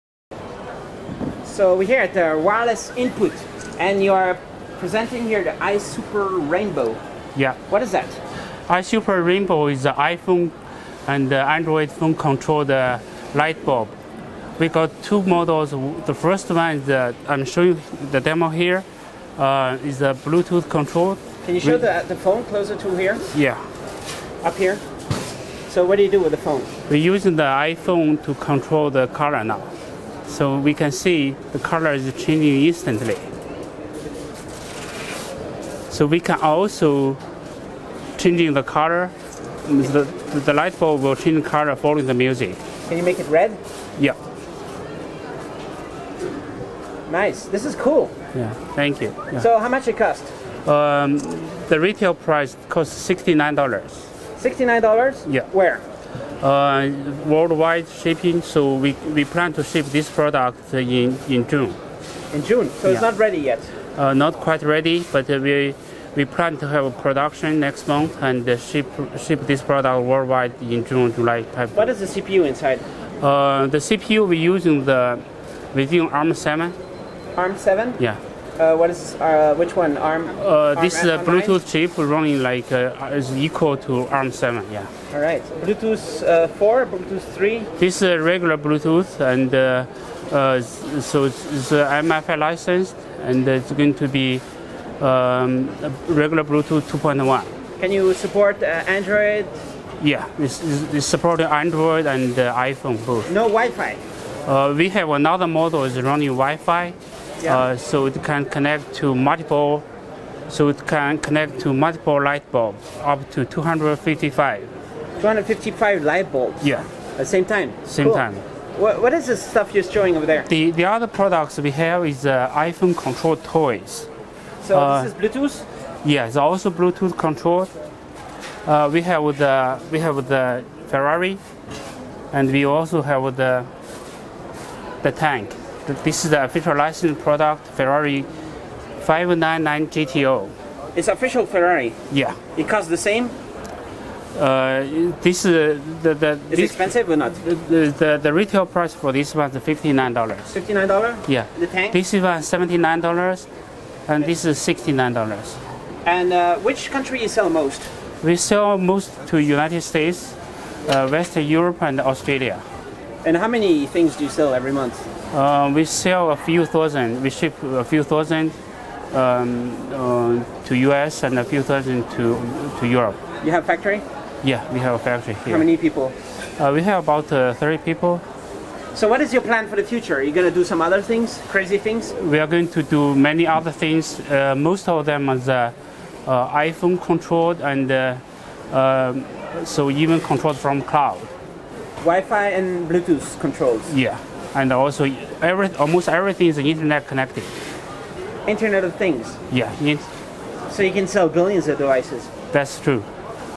Wireless Input iSuper Rainbow, For The Internet of Things
Wireless Input is the creator of iSuper remote controlled products for the "Internet of Things". Most of these products work through Wifi or Bluetooth and they work with Android or iOS devices. iSuper devices primarily targets United States, Western Europe, and Australia. Wireless input uses a moderately size factory employing about 30 people.
Some of the product they create include.
The iSuper rainbow a remote controlled colored lightbulb that works with an Android or iOS device for around 69 USD. The device runs on an unspecified Arm7 processor and uses Bluetooth 2.1. The product will selling worldwide soon.
Other products Wireless Input creates include mobile controlled toys such as a mobile controlled Ferrari for 59 USD, a remote controlled tank 79 USD, and a remote controlled helicopter for 69 USD.

Filmed at the Hong Kong HKTDC Electronics Fair using the Panasonic GH3 camcorder with 12-35mm lense on a Tiffen Steadicam Merlin 2 with vest